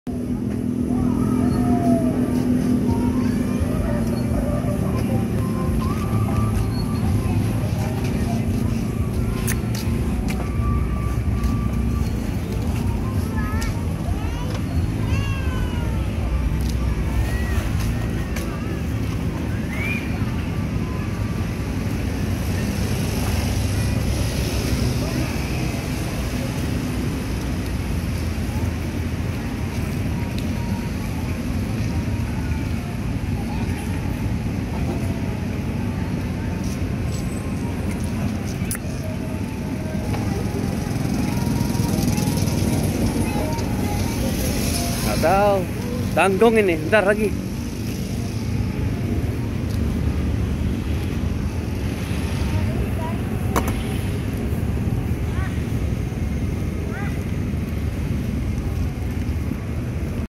Pasar Rakyat Batam ini, Dan Kuliner Petang di daerah Sagulung. Sederhana dan Pasar Rakyat Yang Murah
Kuliner Petang Batam, Salah satu sudut kuliner pinggiran di daerah Sagukung Batam.
kuliner UMKM yg di kelola instansi ini sangat membantu masyarakat sekitaran...